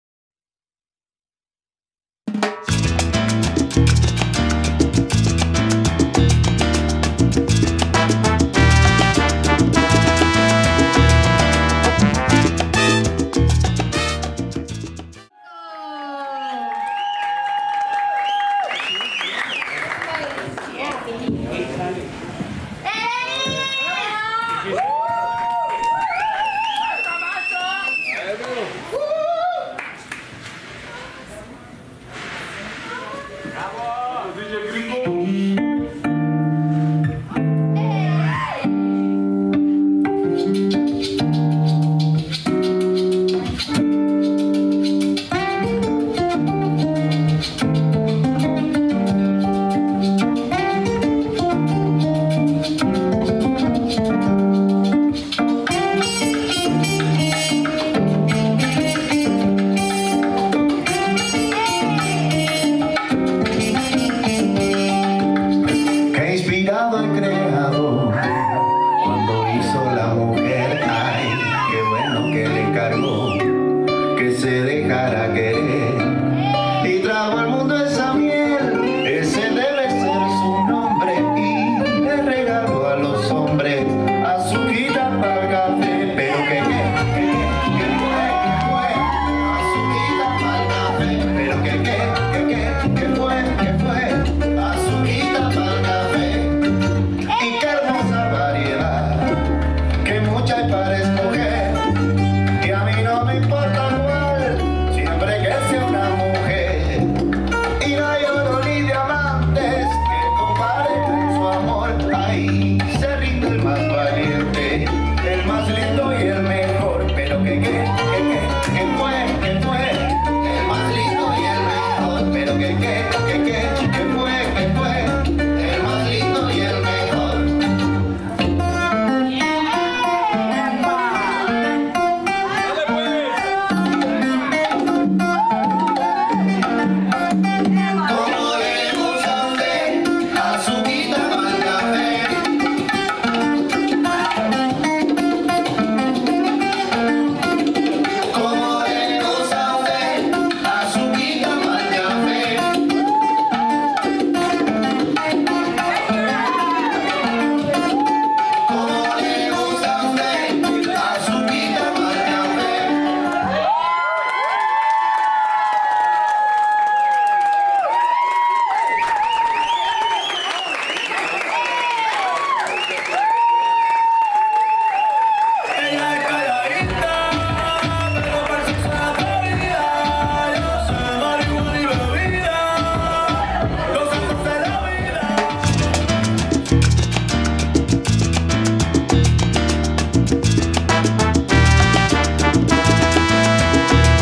La Rumba - 12/8/2023 - Sabor Unico
Sabor Unico performs at La Rumba at Terminal 8 in Raleigh, NC.